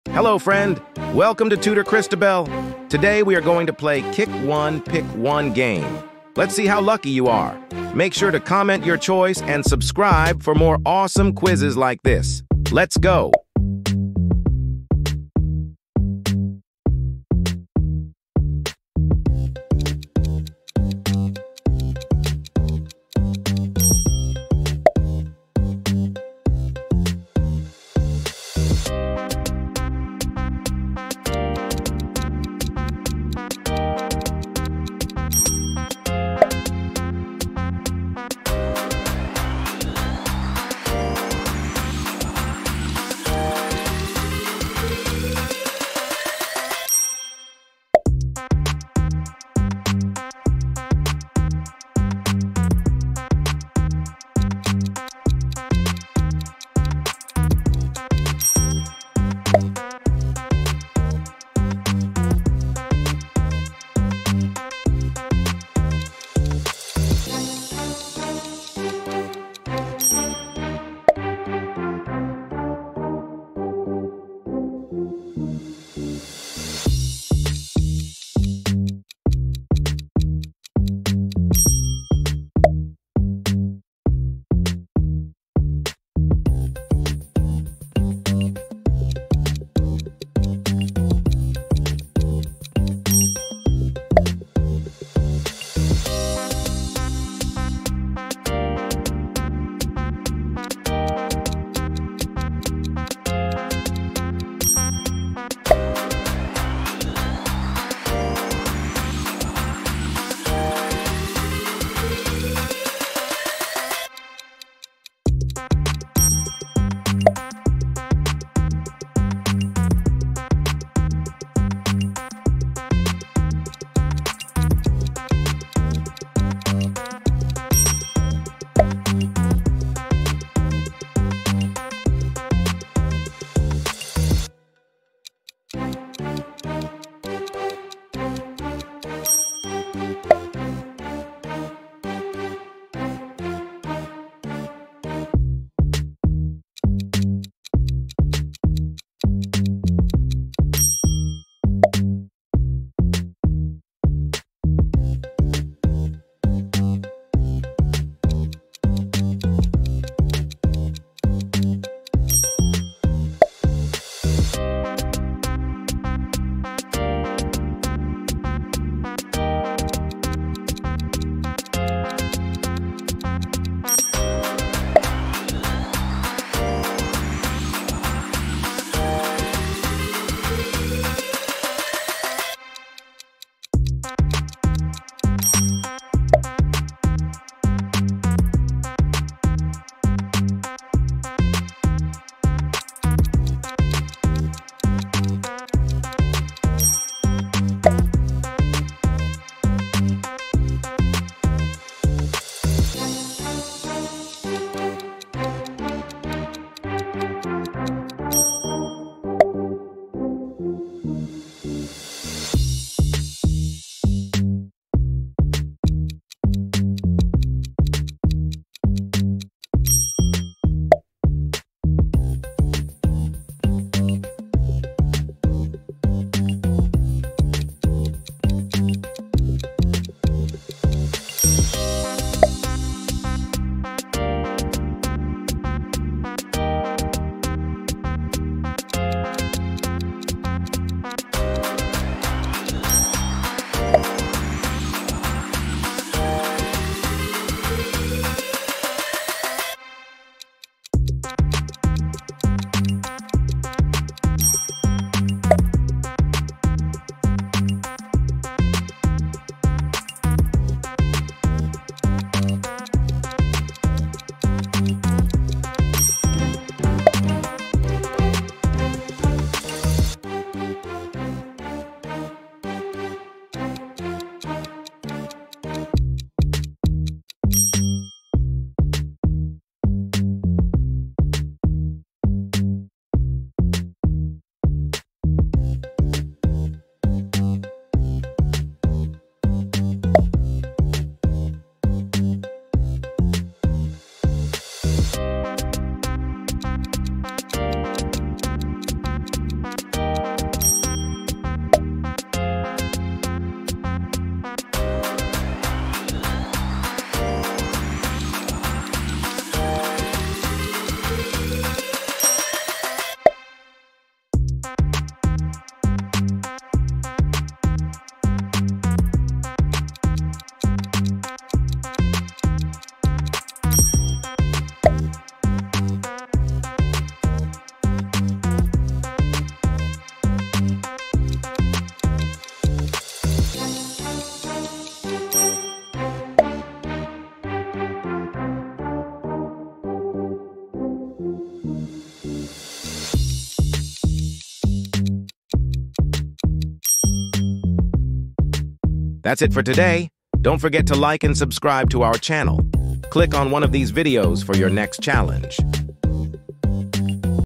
Choose One Button...! - YES or NO
In this video, we're going to be taking a look at a Choose One Button...! This game is a great way to practice your decision making skills.

By playing this game, you'll be able to practice making quick, confident decisions. The goal of this game is to choose one of the two options, and you can't go back after you've made your choice. This is a great way to practice your decision making skills, and it's also a lot of fun!

So let's get started, and choose one button...!